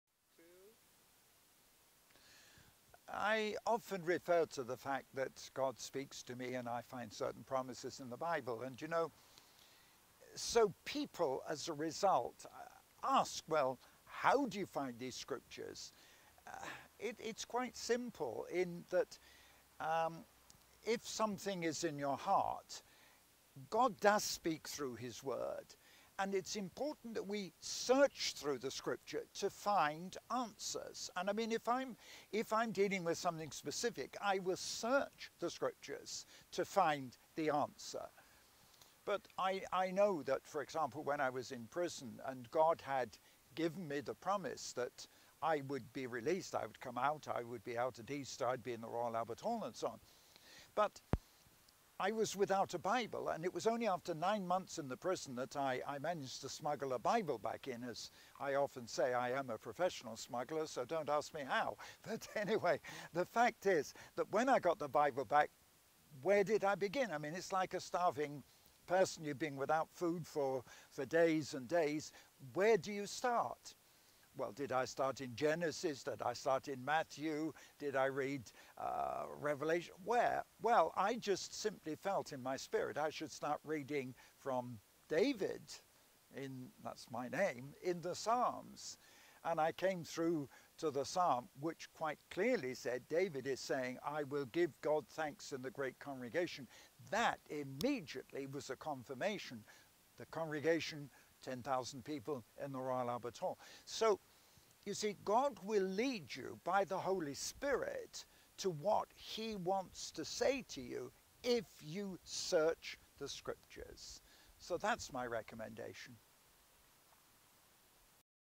How To Find the Promises of God
God speaks to us through the Bible. It is important that we search the Scripture to find the answers we are looking for. If I am facing a specific situation, I will search the Scriptures for an answer to that problem. If something is in your heart, then God will lead you by the Holy Spirit to what He wants to say to you, if you will search the Scriptures.